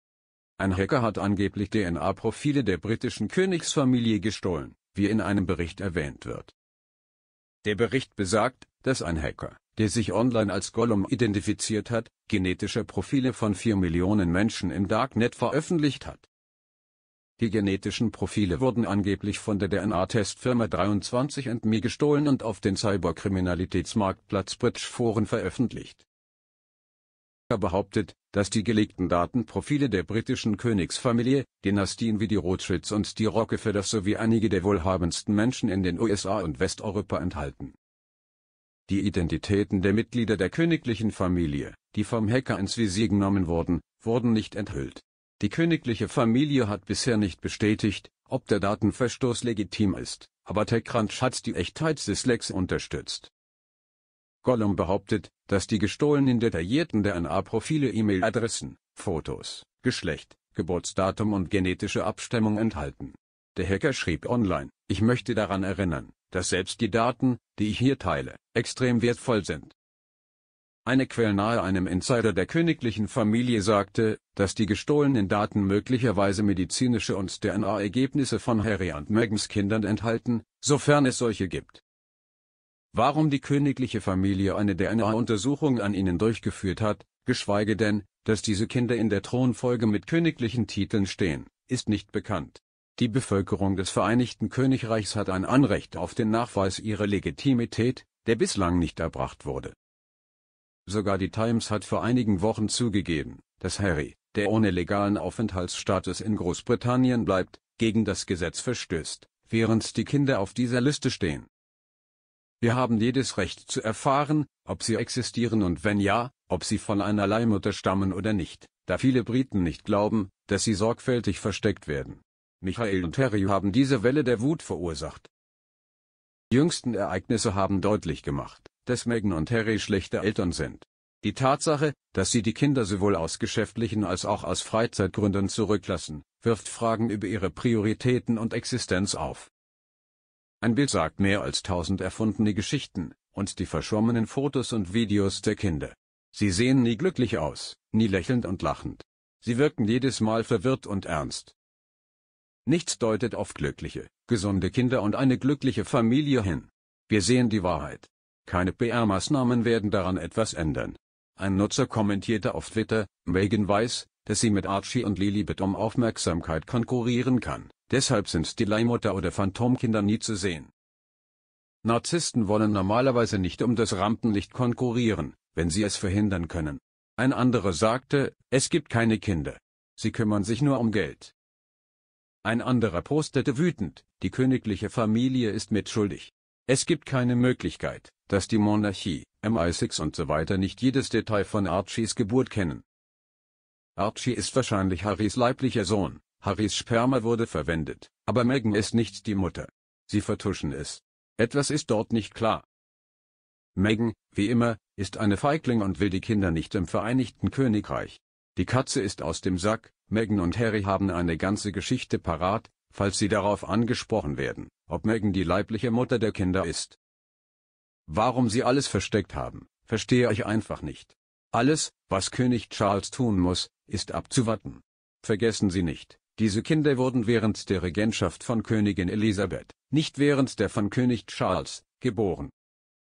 Meg Wird Blass, Als Hacker Die Aufzeichnungen Von Archie Und Iilibet Preisgeben: Sie Sind Gefälscht
Einem Bericht zufolge hat ein Hacker DNA-Profile der britischen Königsfamilie gestohlen.
Dem Bericht zufolge hat ein Hacker, der sich online als Golum identifizierte, die genetischen Profile von vier Millionen Menschen im Dark Web preisgegeben.